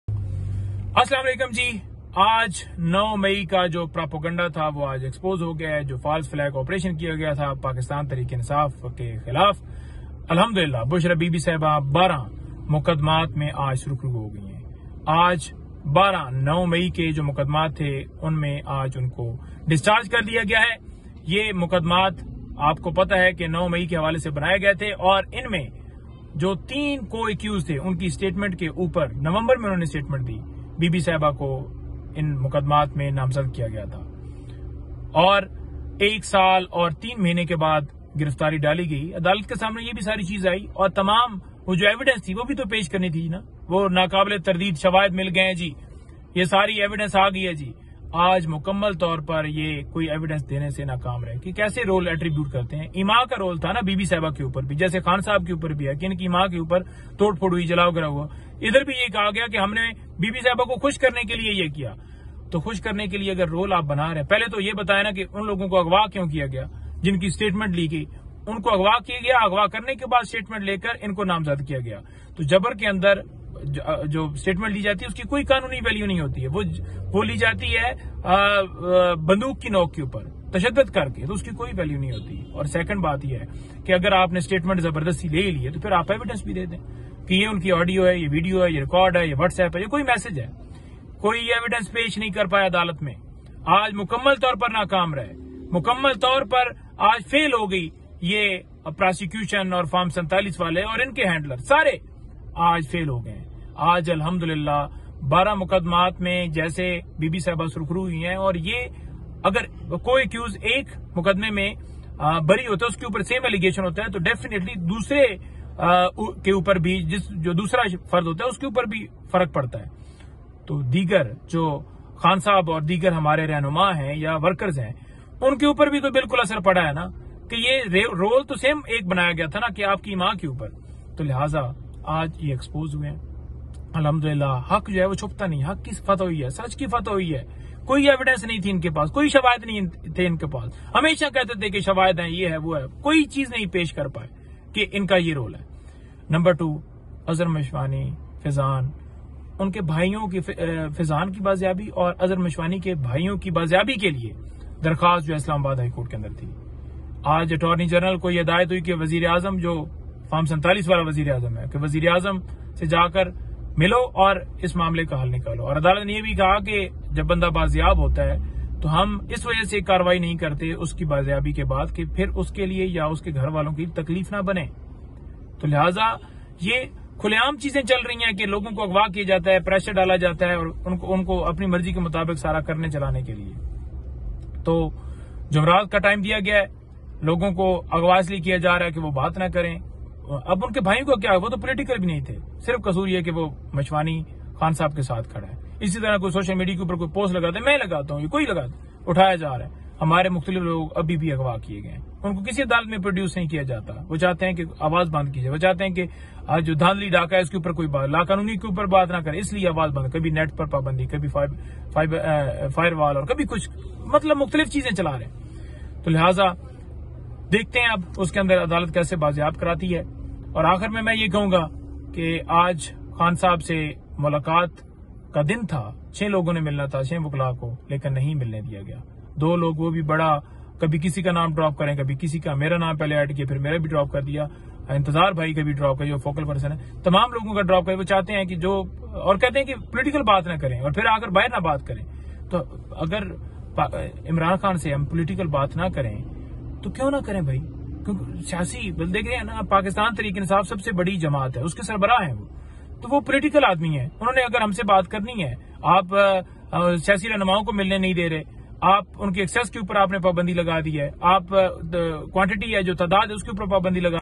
الحمدوللہ بشری بی بی نو مئی کے مقدمات میں باعزت ڈسچارج ہو گئی
الحمدوللہ بشری بی بی نو مئی کے مقدمات میں باعزت ڈسچارج ہو گئی،اظہر مشوانی کے بھائیوں کو بازیاب کرنے کی درخواست پر جمعرات تک عدالت کی طرف سے مہلت دی گئی ،خان صاحب سے وکلاء کی ملاقاتوں پر بھی پابندی .